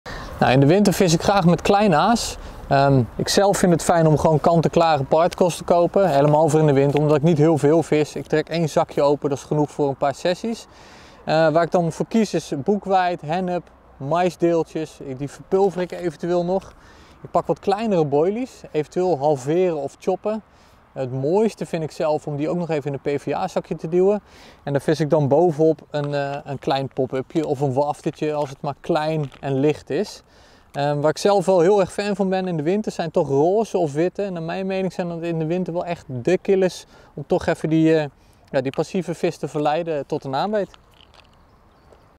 Beste voer tijdens het karpervissen in de winter!
Peter uit ons filiaal in Lelystad laat zien welk voer zijn voorkeur heeft tijdens het karpervissen in de winter!